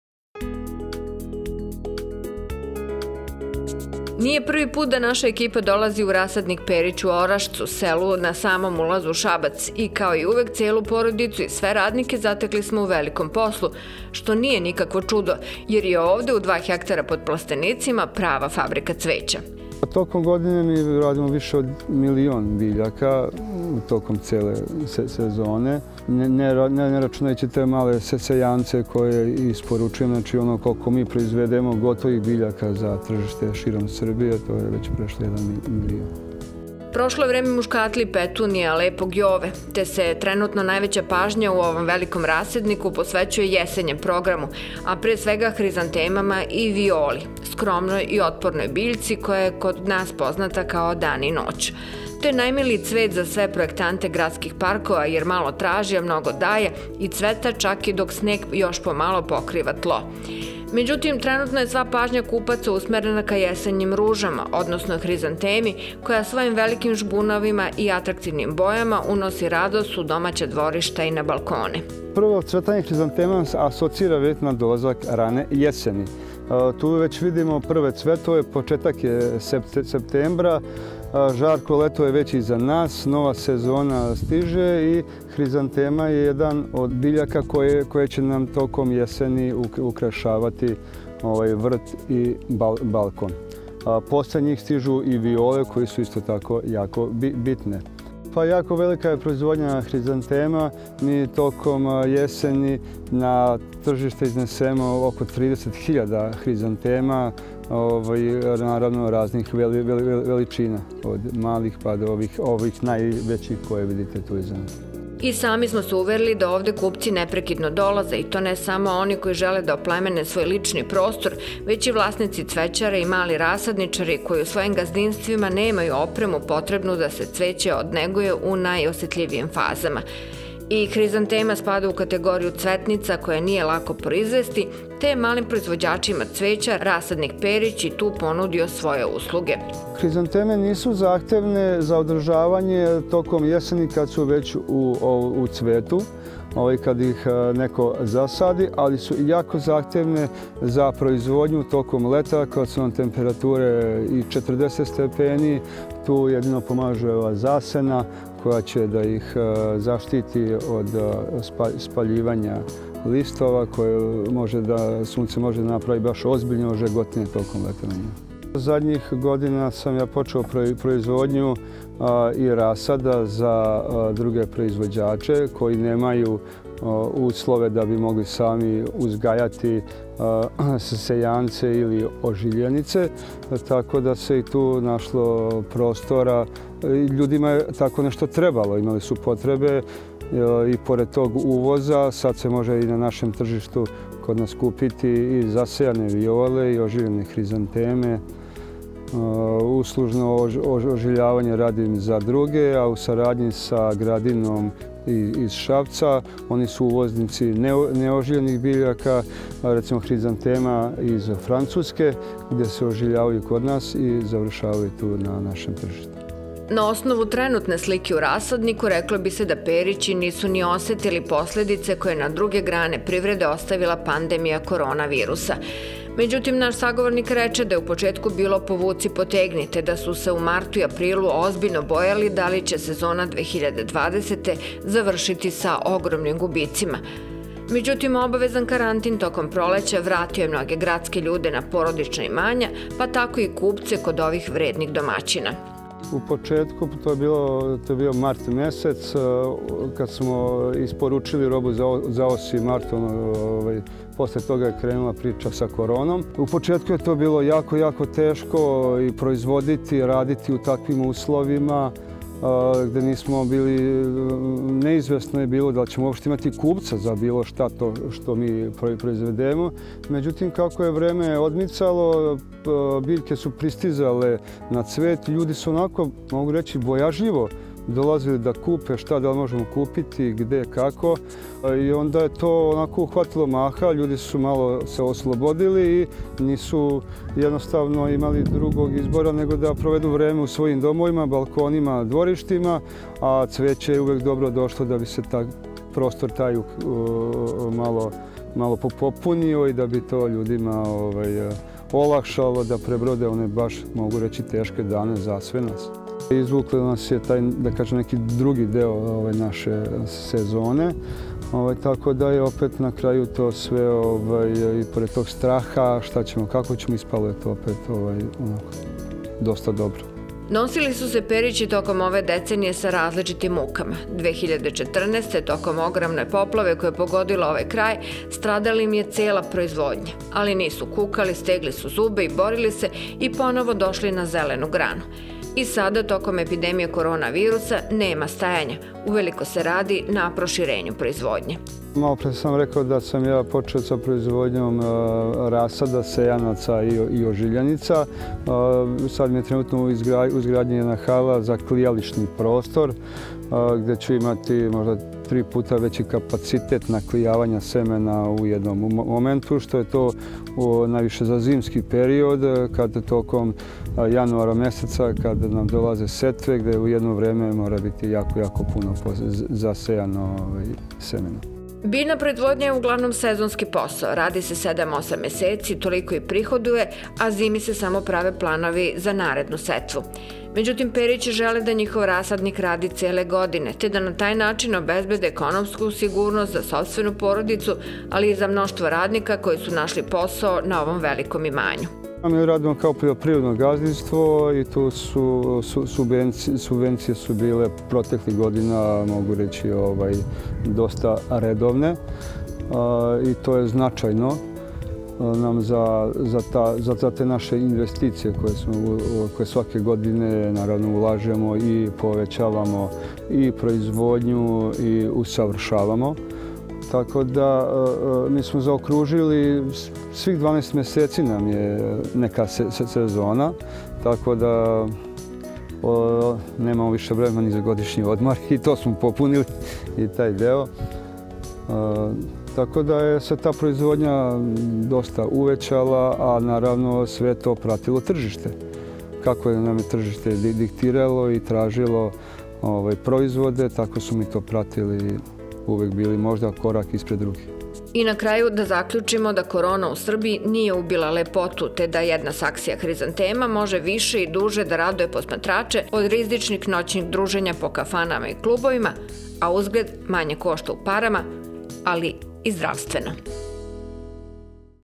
HRIZANTEMA - jesenja KRALJICA dvorišta
Gde je prestonica hrizanteme - jesenje kraljice dvorišta?
Sagovornik: Dejan Perić - Rasadnik Perić, Orašac
Facebook : Farma emisija o poljoprivredi